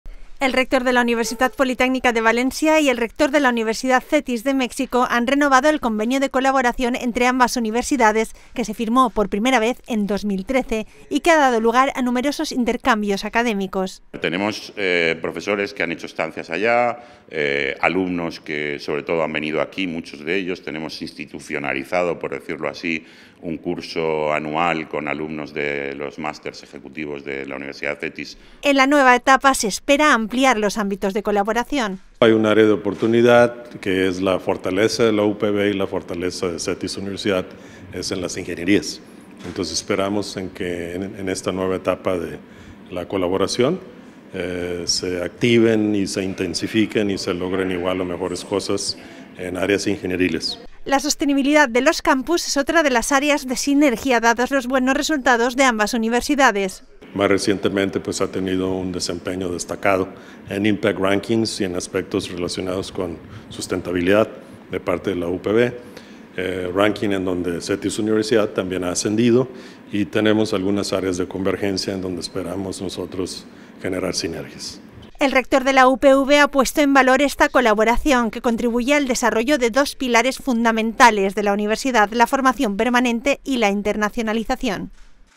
Universidad CETYS de México - Noticia @UPVTV; 17-07-2023
La UPV y la universidad mexicana colaboran en actividades de formación permanente desde 2013.